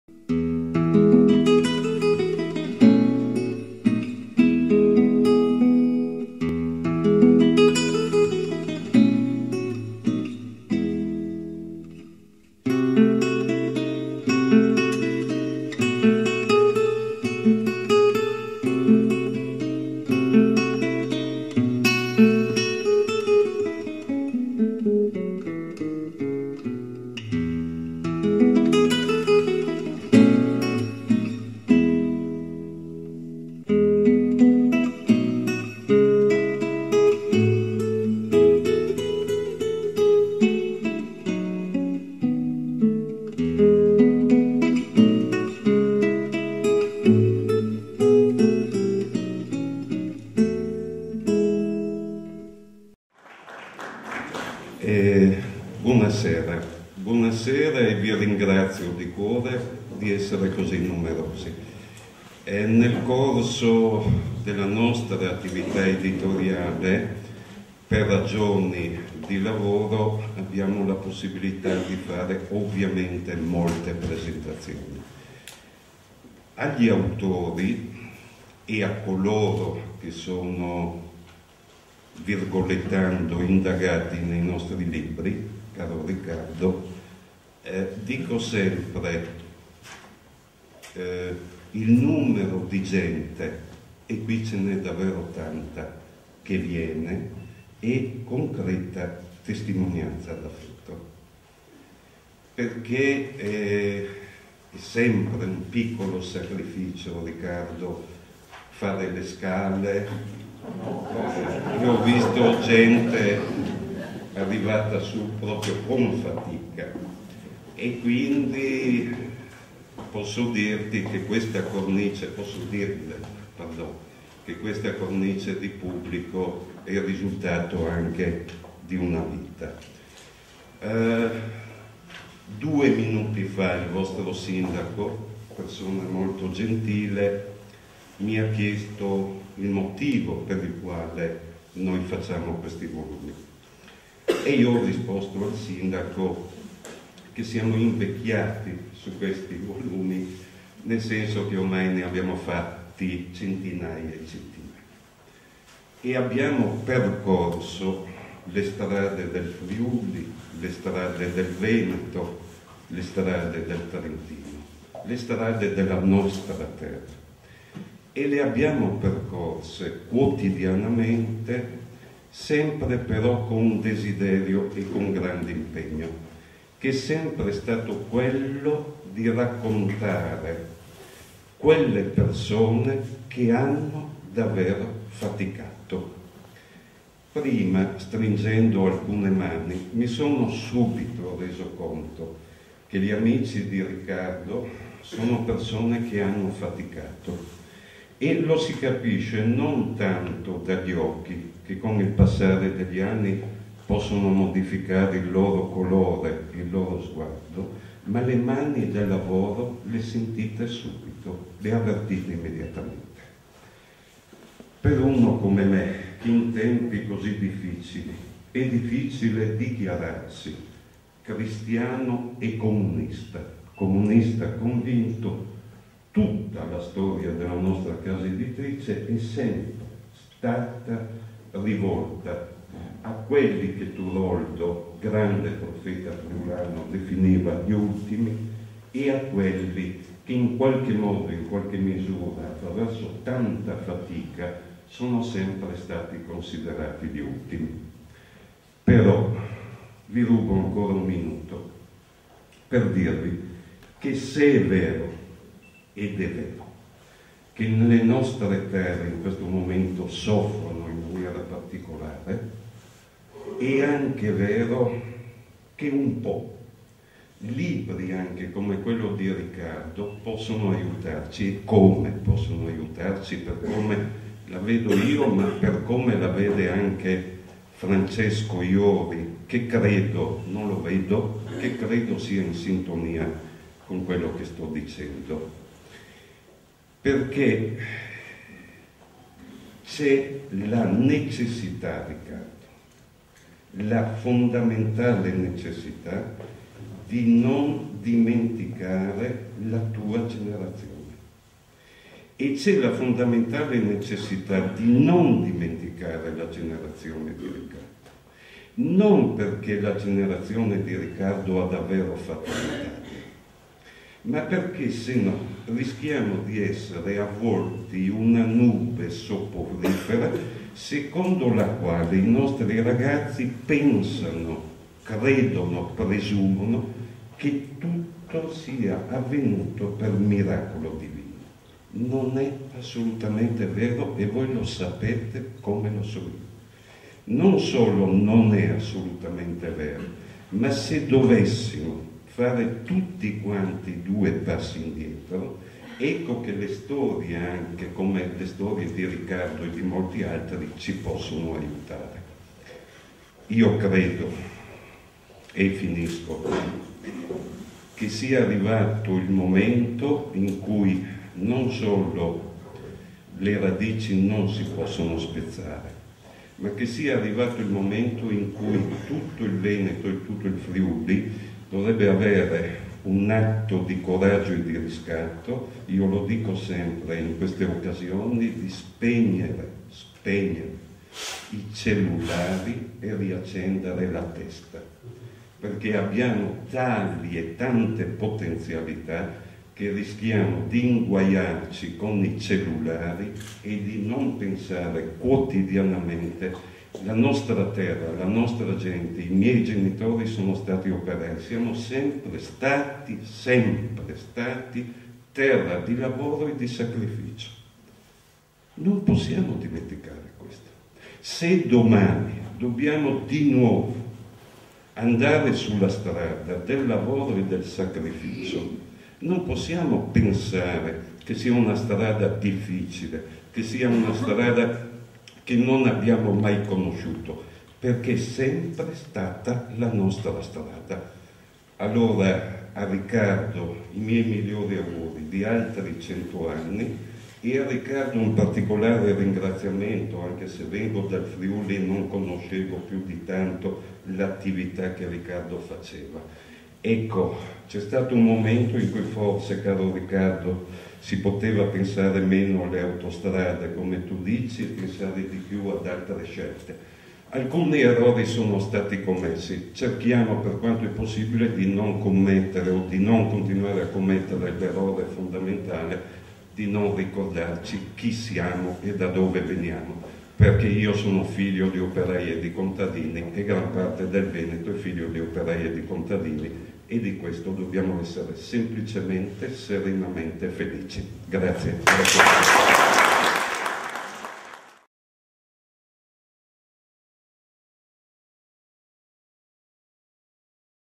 "L'ultimo dei barcàri..." un libro di Francesco Jori, interviene il Dott. Giovanni Santarossa
By FergidMultimedia 2009 [italiano-english text below] Nella suggestiva cornice dell'antica e splendida Villa Selvatico Sartori a Battaglia Terme, è stato presentato Sabato 21 Marzo 2009 il libro di Francesco Jori "L'ultimo dei bàrcari: Riccardo Cappellozza, una vita sul fiume", edizione Biblioteca dell'Immagine. Alla presentazione, guidata dal Dott. Stefan Marchioro Direttore Azienda Turismo Padova Terme Euganee, sono intervenuti l'editore Giovanni Santarossa, il Sindaco di Battaglia Terme Dott.ssa Velia Bevilacqua, Flavio Manzolini Assessore Provinciale allo Sviluppo Economico e alle Identità Venete, Francesco Vallerani Docente di Geografia all'Università Ca' Foscari di Venezia, Riccardo Cappellozza fondatore e anima del Museo Civico della Navigazione Fluviale, il Dott. Elio Franzin, il Dott. Michele Cortellazzo docente della Facoltà di Lettere e Filosofia dell'Università di Padova e a chiusura della presentazione, l'autore dell'opera Dott. Francesco Jori. In questo video l'intervento dell'editore Dott. Giovanni Santarossa.

[english] In the backdrop of the ancient and splendid Villa Sartori Selvatico a Battaglia Terme, was presented Saturday, March 21, 2009 by Francesco Jori book "The last of the boatmen: Riccardo Cappellozza, a life on the river ", edition of Image Library. To presentation, led by Dr. Stefan Marchioro Director Corporate Turismo Padova Terme Euganee, have the publisher Giovanni Santarossa, the Mayor of Battaglia Terme Dr. Velia Bevilacqua, Flavio Manzolini Provincial Economic Development and Identity to the Venetian, Francesco Vallerani Professor of Geography at the University Ca' Foscari Venice, Riccardo Cappellozza founder and soul of the Museum of River Navigation, Dr. Elio Franzin, Dr. Michele Cortellazzo lecturer of the Faculty of Arts and Philosophy, University of Padua and close the presentation, the author of Dr. Francesco Jori. In this video the intervention's Dr. Giovanni Santarossa.